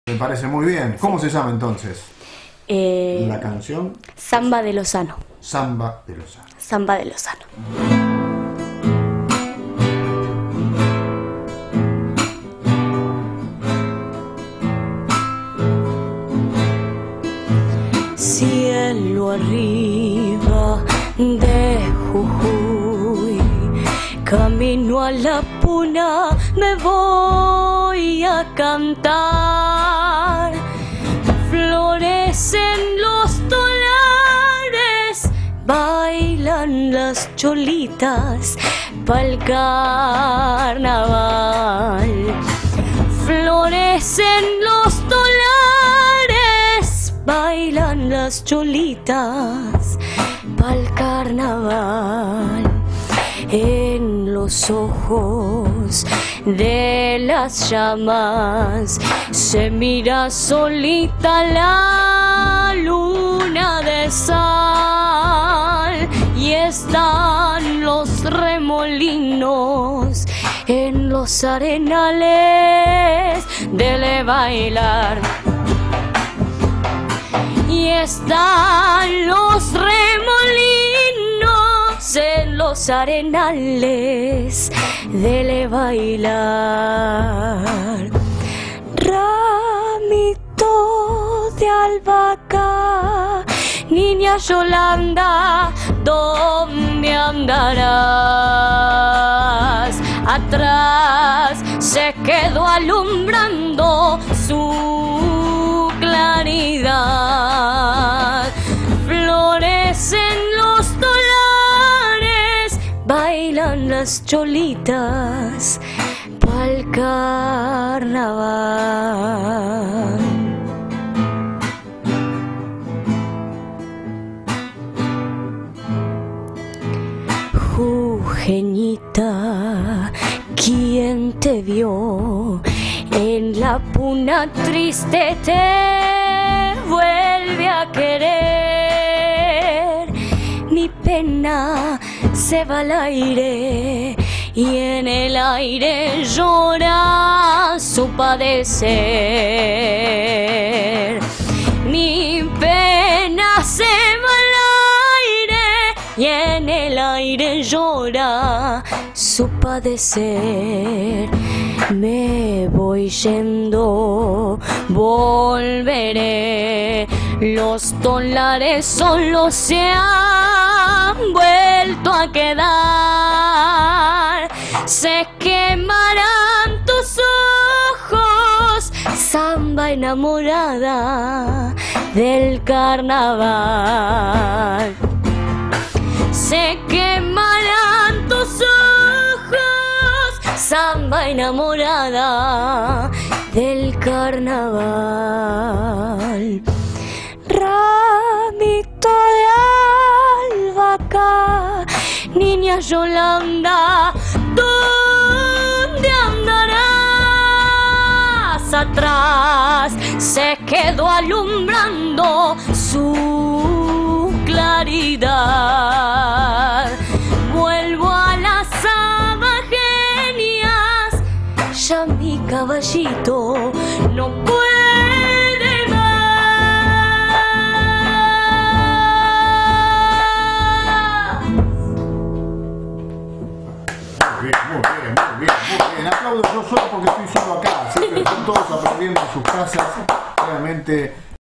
Abriendo EN VIVO Belén Laurenz en DEMOS Radio Visual
La mejor forma de festejar nuestra Fecha Patria, Mario Oneto recibe a Belén Laurenz, acompañada por la guitarra de Brian Navarro y el bombo del Bombisto y Luthier Miguel Navarro; finalizando su gira por Buenos Aires y antes de regresar a su Olavarria natal, se presentan en vivo en DEMOS Radio Visual .